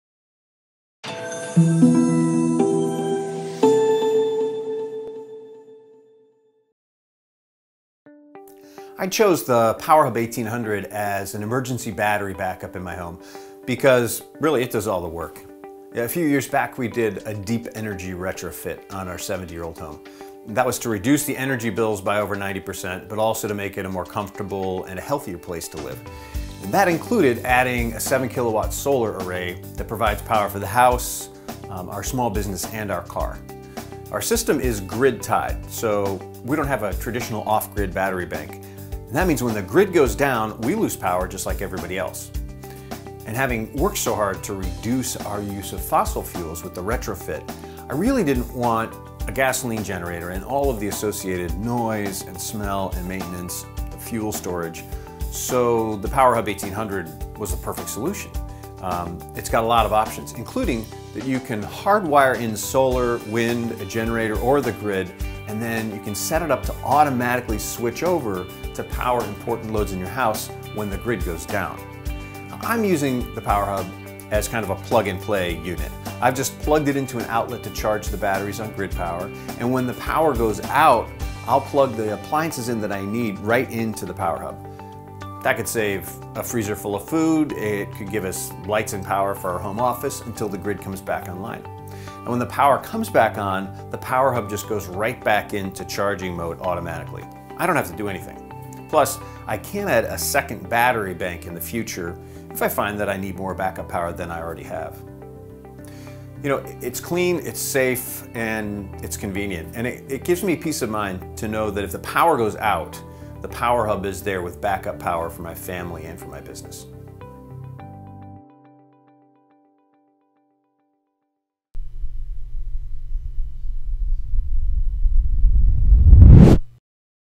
Jeff Wilson on the Xantrex Powerhub 1800 inverter / battery box in his green retrofitted house
HGTV and DIY network host Jeff Wilson recently installed the Xantrex Powerhub 1800 as part of a deep energy retrofit done to their 70 year old home.